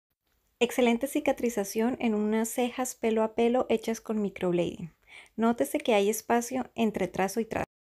Resultado: cejas cicatrizadas/sanadas con microblading pelo a pelo
Cejas hechas con microblading 30 dias después, por qu es necesario un retoque en microblading?